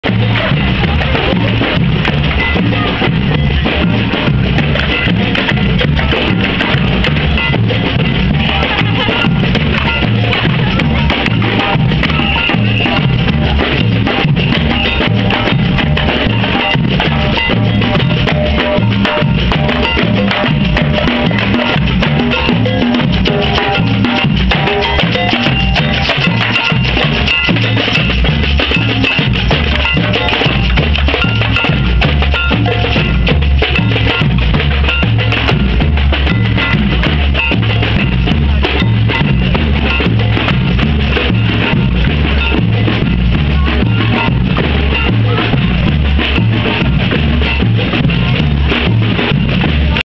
Sketty Park Carnival 2012
clip of Tim the Guitar Man at the Sketty Park Carnival 2012